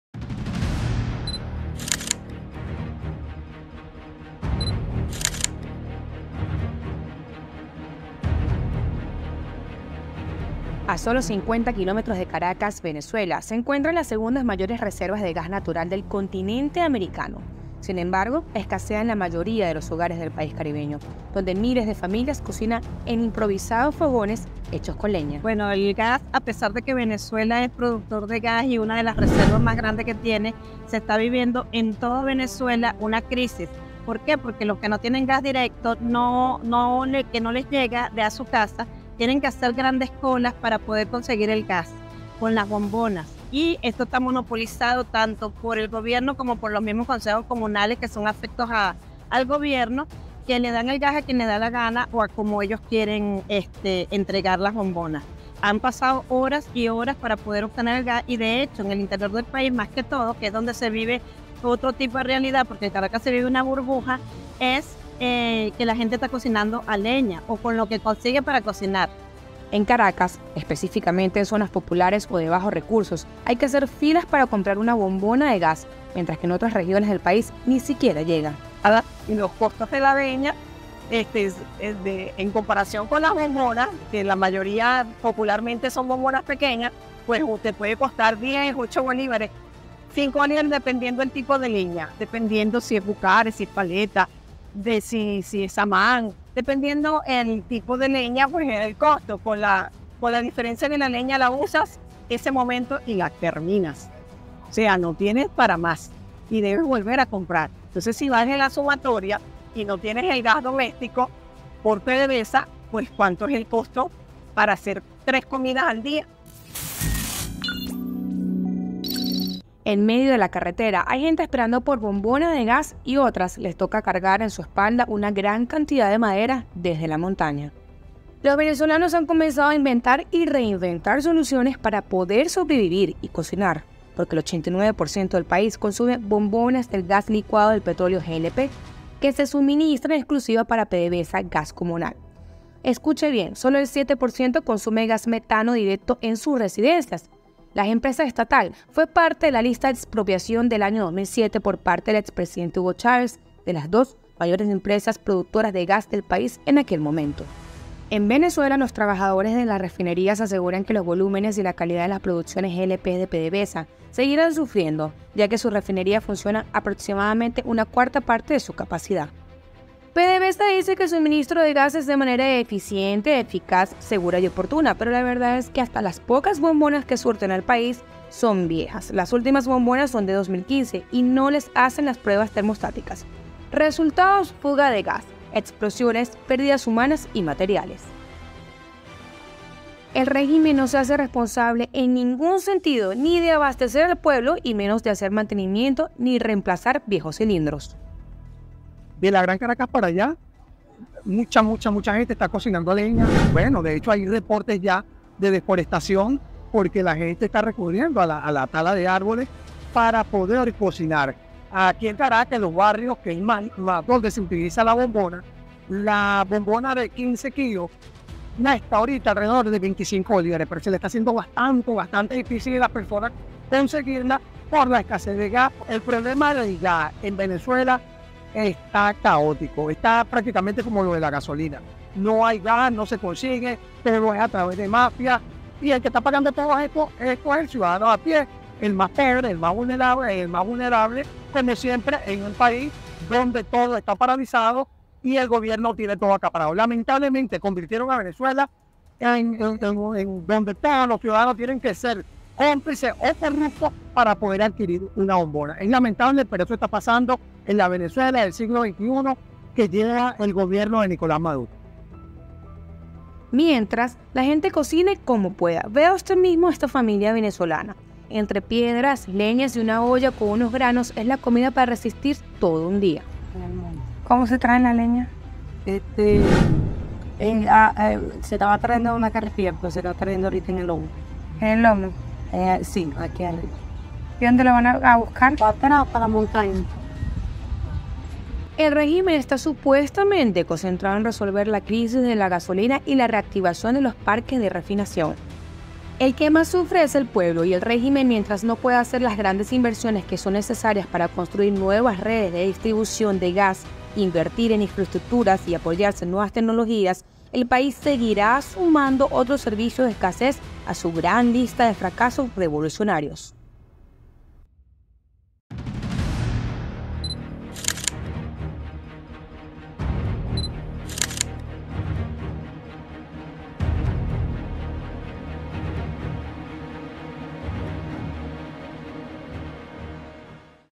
Venezuela retorna a la cocina de leña por falta de gas | El Tiempo
El régimen de Nicolás Maduro culpa la escasez y otros problemas con los servicios básicos a las sanciones internacionales que impiden modernizar los sistemas y darles el mantenimiento requerido.

En Caracas, específicamente en zonas populares o de bajos recursos, hay que hacer filas para comprar una bombona de gas, mientras que en otras regiones del país ni siquiera llega.

Mira todos los programas especiales de Dossier Venezuela y entérate, sin censura de lo que está pasando en Venezuela